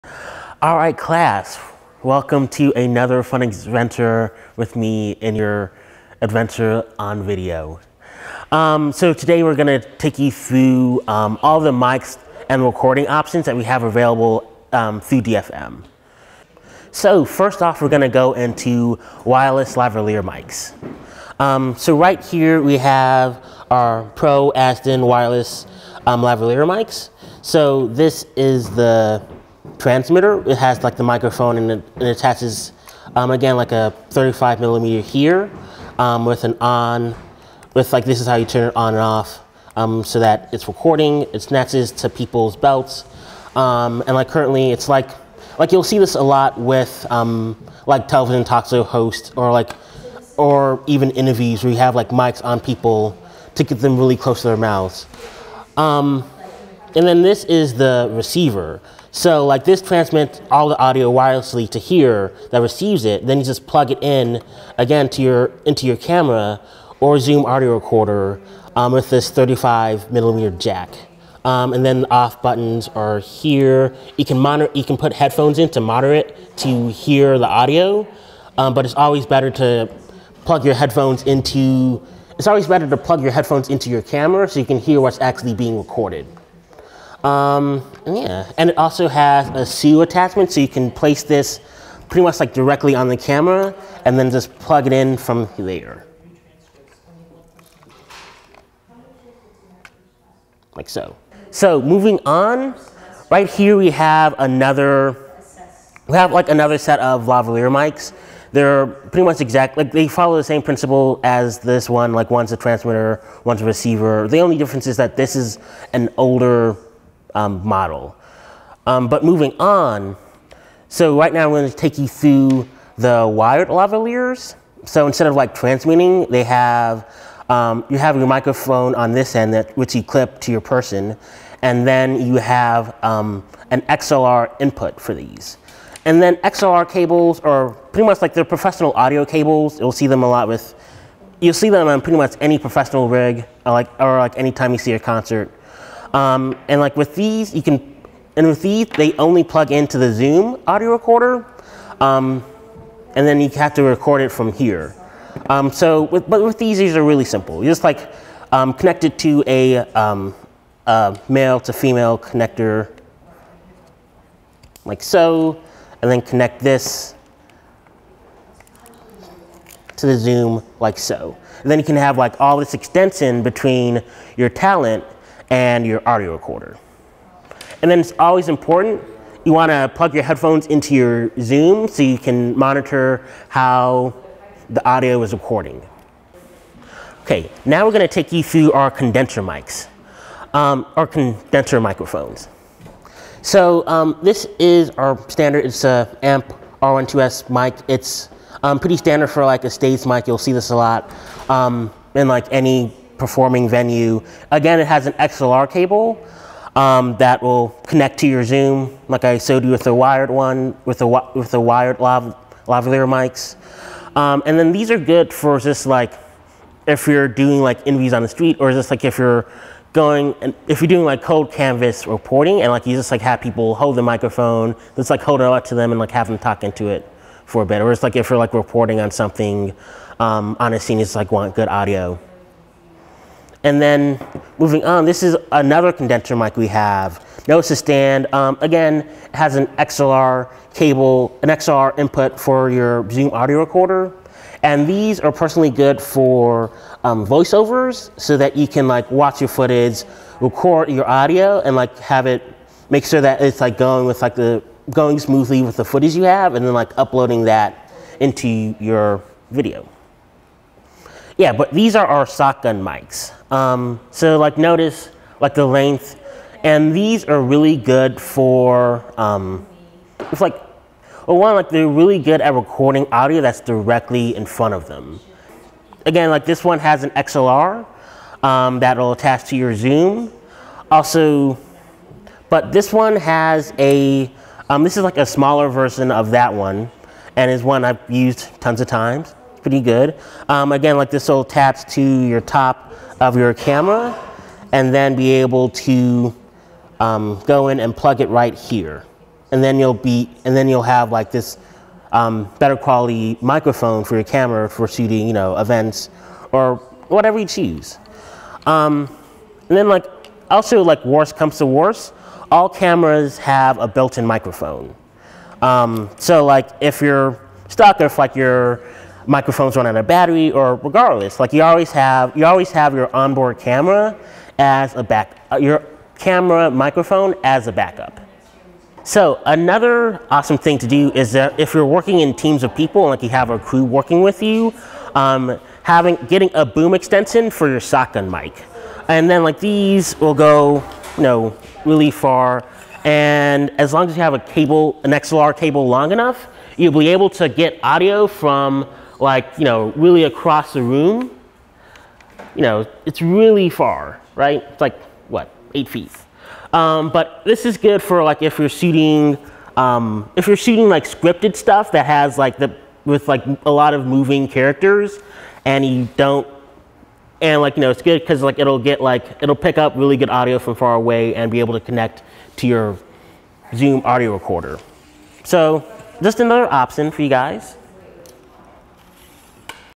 DFM Video - Audio Overview
overview of microphones and how to use them.